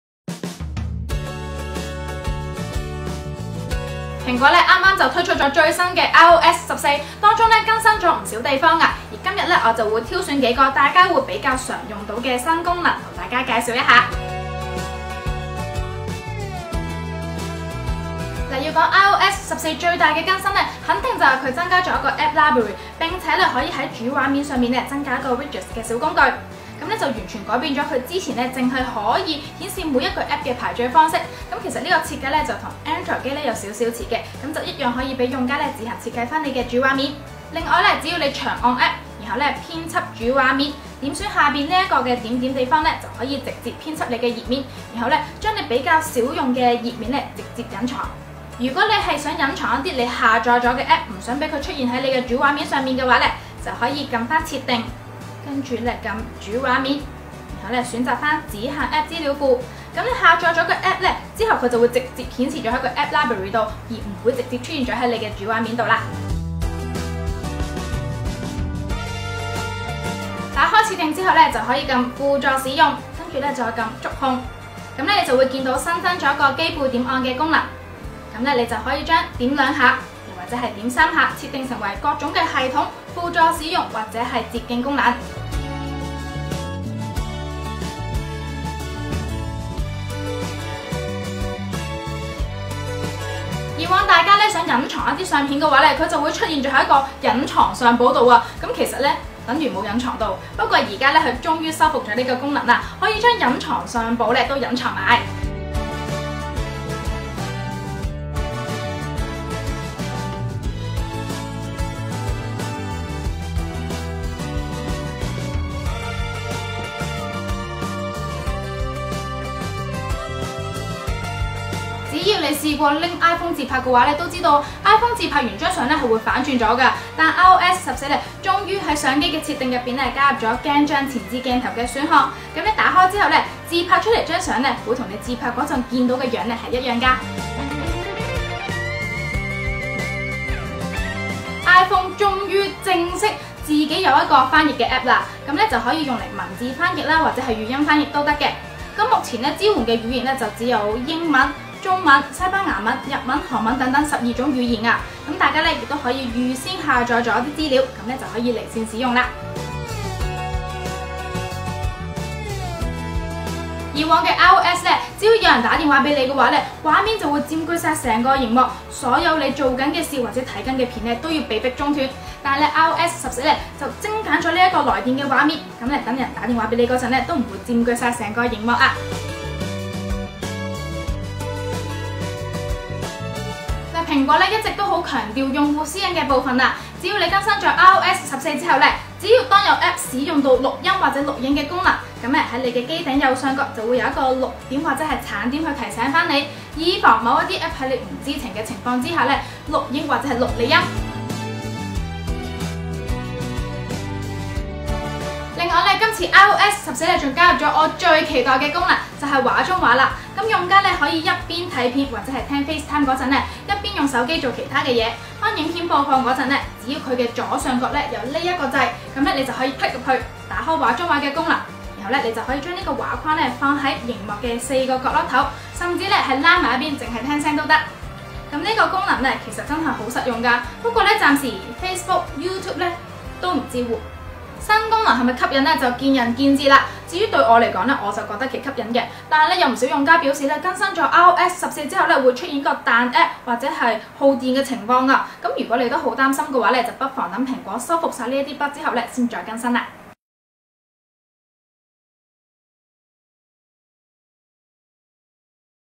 【社交班】iOS14 8大實用新功能！
iOS 14面世喇！每次iOS更新之後都會岀現一啲漏洞，但都係有唔少用家會選擇即刻更新今次嘅iOS14有啲咩新功能吸引到一眾用家？ 實用性有幾高？又係咪值得即刻更新？即刻去片啦！

*蘋果日前已經推出最新嘅iOS14.0.1
全方位緊貼《U Magazine》社交平台